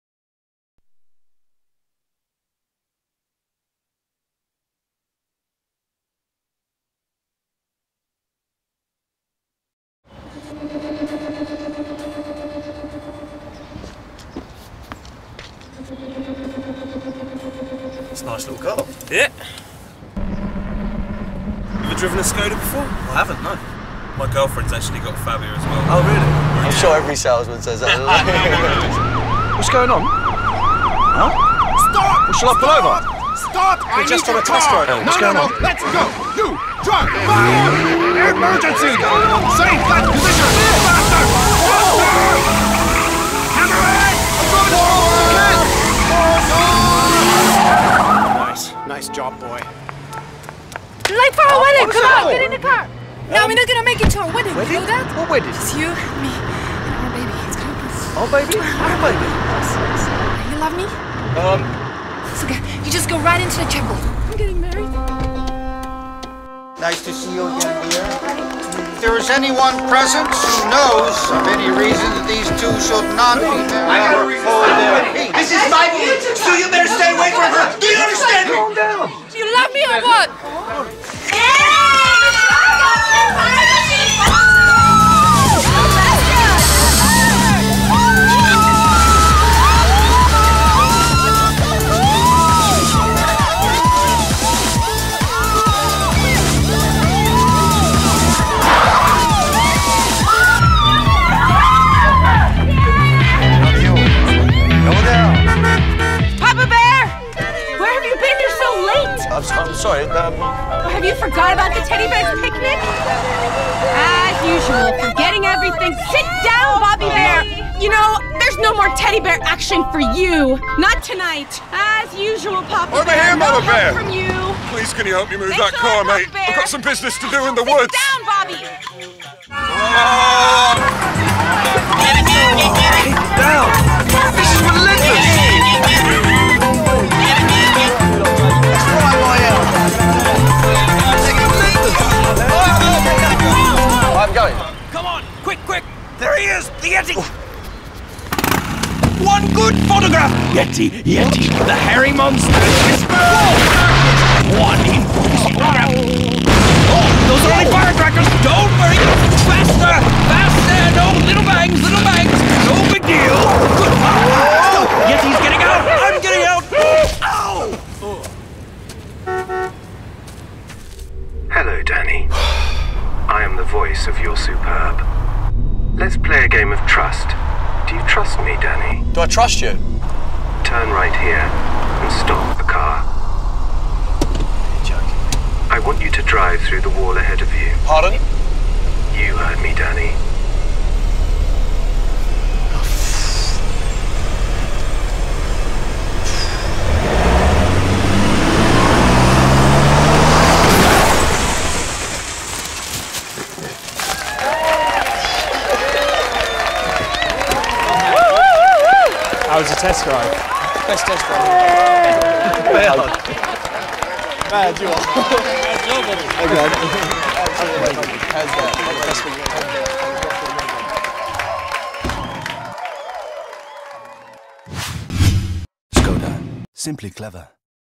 SKODA Тест-драйв модельного ряда автомобилей 2016-2017года
Уважаемые зрители этот сумасшедший Тест-драйв показывает, как  модельный ряд автомобилей SKODA готовы к любой ситуации.